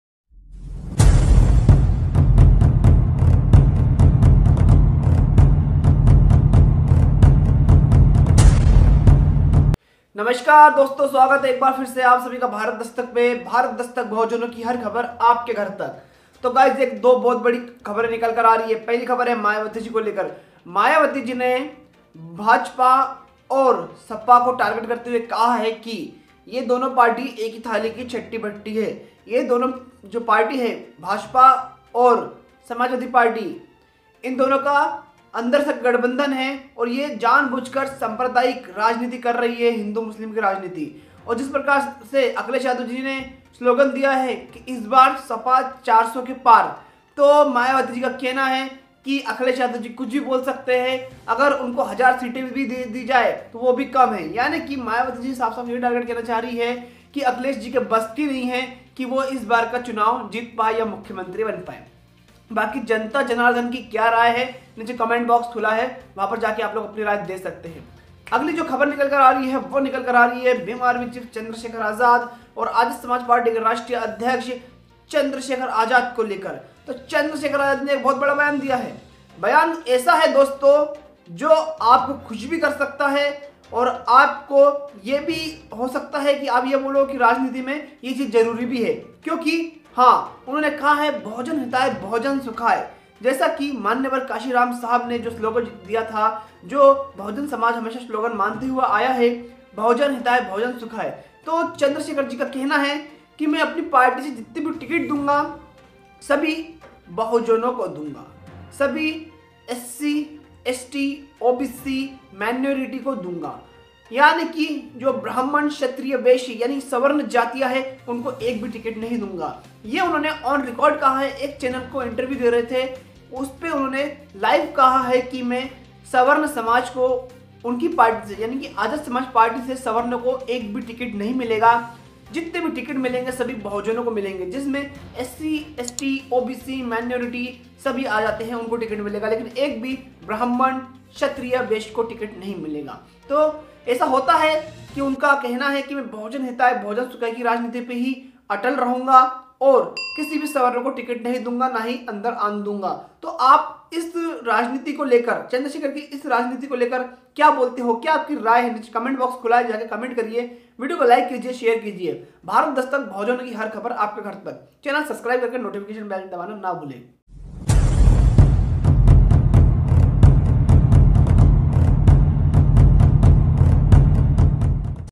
चंद्रशेखर रावण का बड़ा ऐलान किसी सवर्ण को पार्टी टिकिट नही देंगे
About Video👇👇
In Thise Video Many News About Bahujan Community people . Bharat Dastak Giving Everyday New Video About Bahujan Community News .